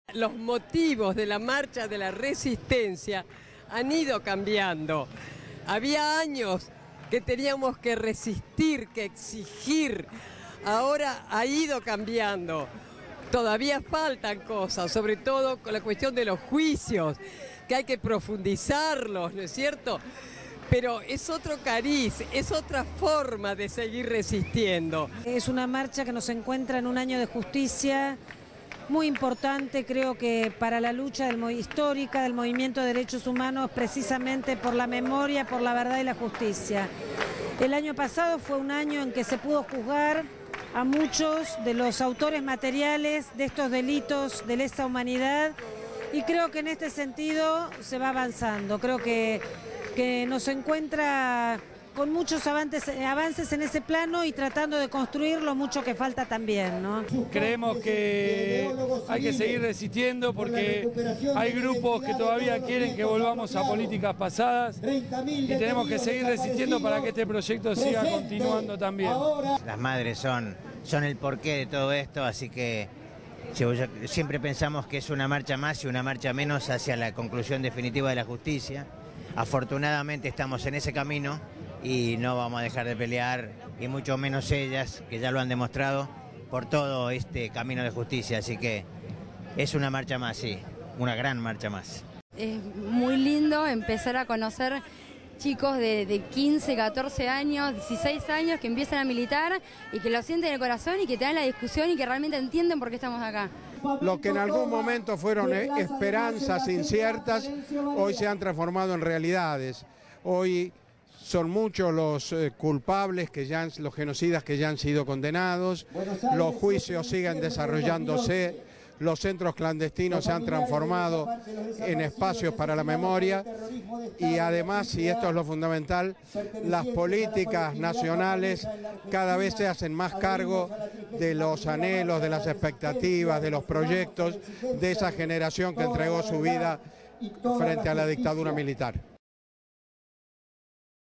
Se realizó la 31º Marcha de la Resistencia.
Abuelas y Madres de Plaza de Mayo-Línea Fundadora encabezaron hoy una nueva Marcha de la Resistencia, acompañada por una amplia presencia de organismos de derechos humanos, al conmemorarse el 30º aniversario de la primera vez que un grupo de Madres caminó durante veinticuatro horas alrededor de la Pirámide de Mayo para reclamar por sus hijos desaparecidos.

Bajo la consigna "Toda la Verdad, Toda la Justicia", la Plaza de Mayo fue el escenario central de un nuevo pedido de juicio y castigo para todos los genocidas, cómplices civiles e ideólogos de la última dictadura militar, para todos los crímenes de la Triple A, por la apertura de todos los archivos, la restitución de la identidad de todos los nietos, por la aparición con vida de Jorge Julio López y la libertad de Karina Germano.